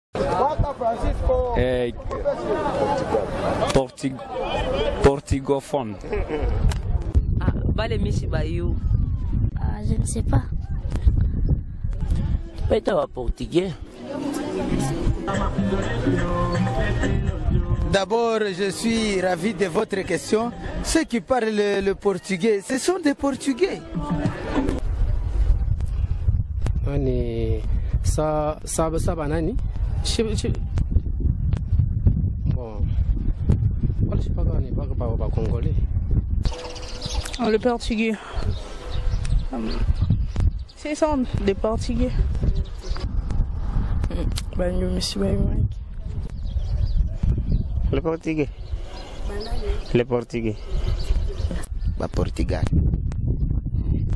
COMMENT APPEL-T-ON LES HABITANTS DU PORTUGAL
La population de kasumbalesa reagit a la question de jambo krtv au sujet de habitants du portugal.